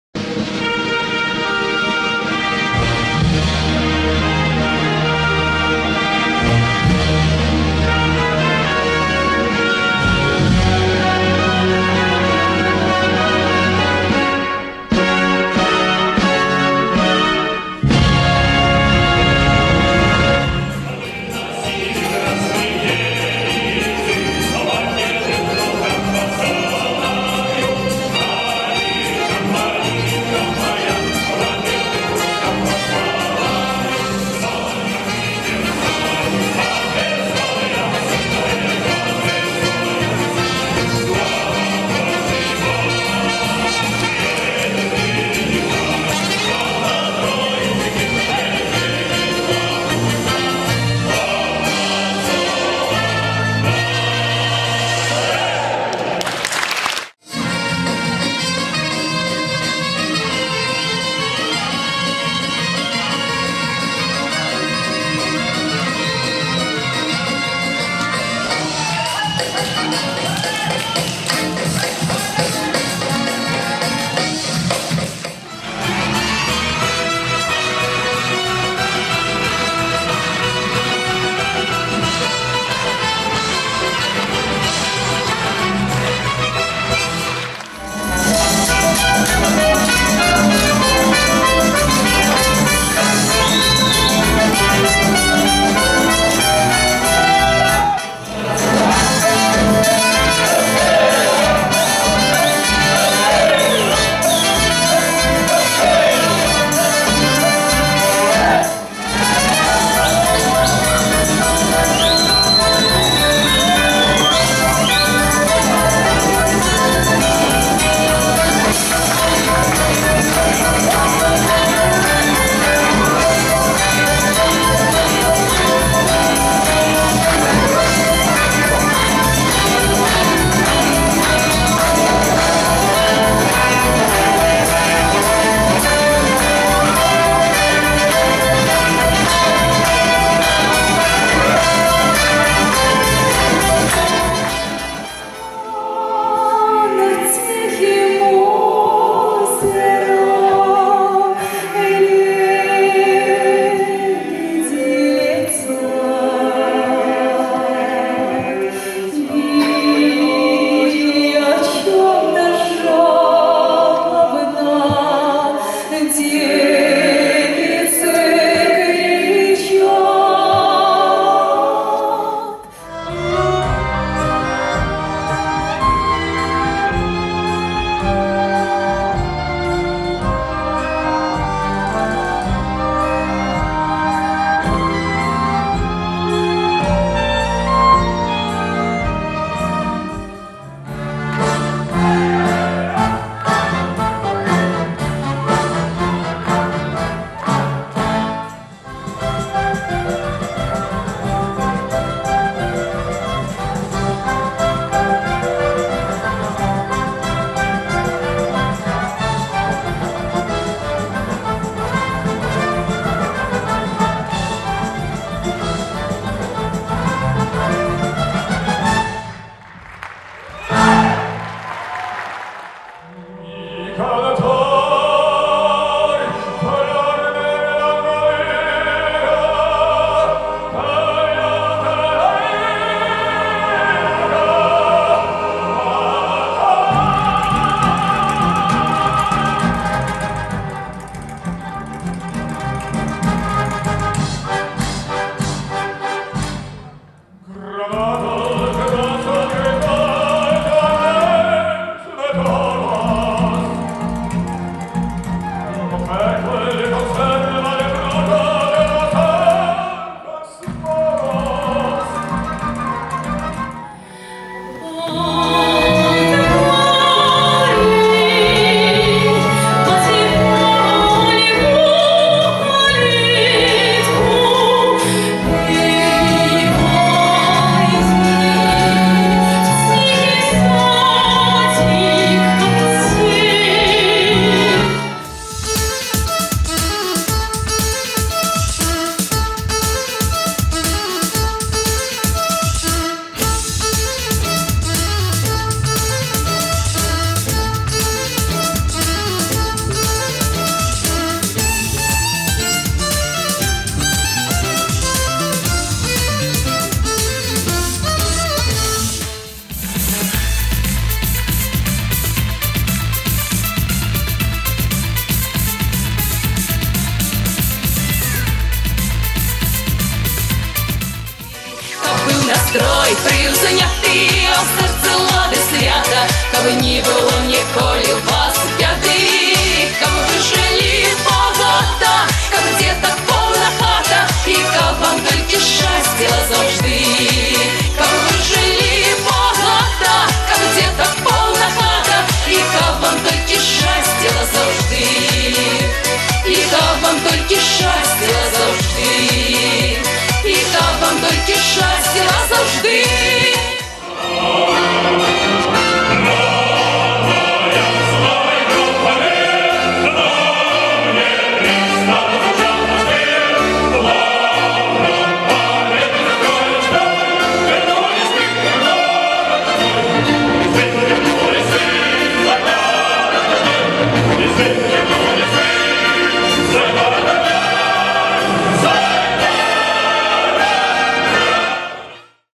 ENSEMBLE MUSIQUE CHANTS DANSES ACADEMIE MILITAIRE DE LA REPUBLIQUE BIELORUSSE
EXCEPTIONNEL DANS LA REGION DE BETHUNE DU 29 AOUT 2014 AU 07 SEPTEMBRE 2014 INCLUS.